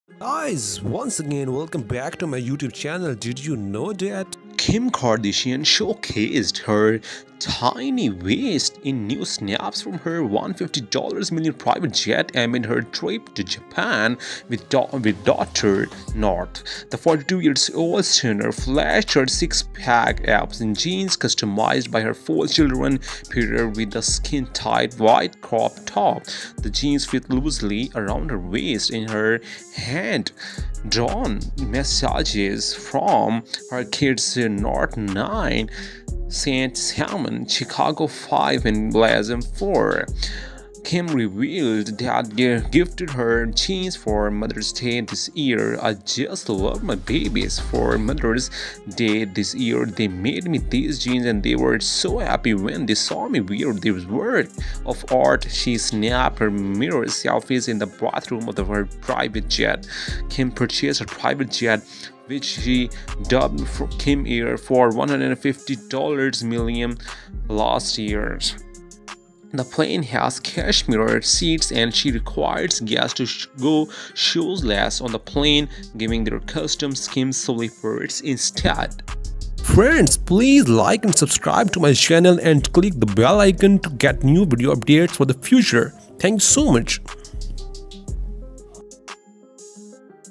Kim Kardashian Shows Off Super Thin Waist as she poses inside her $150M private jet | Holly Set
Kim Kardashian showcases her TINY waist in skintight crop top and jeans customized by her four children... as she poses inside her $150M private jet